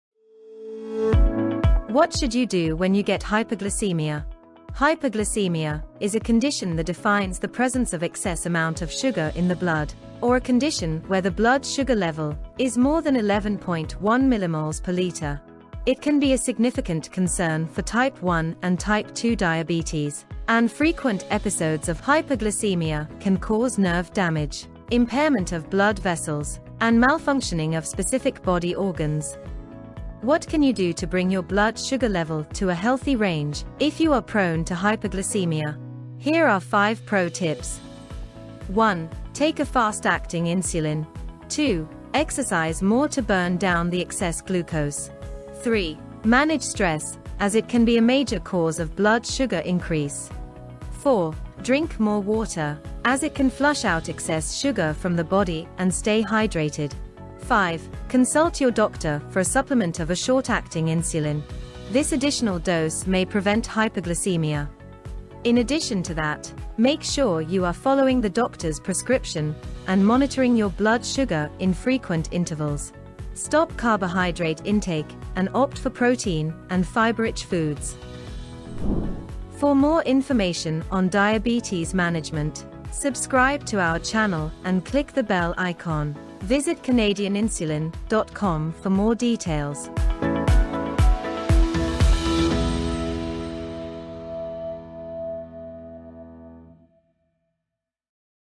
5 Tips to Treat Hyperglycemia
One. Take a fast-acting insulin.

Two. Exercise more to burn down the excess glucose.

Three. Manage stress as it can be a major cause of blood sugar increase.

Four. Drink more water, as it can flush our excess sugar from the body and stay hydrated.

Five. Consult your doctor for a supplement of a short-acting insulin. This additional dose may prevent Hyperglycemia.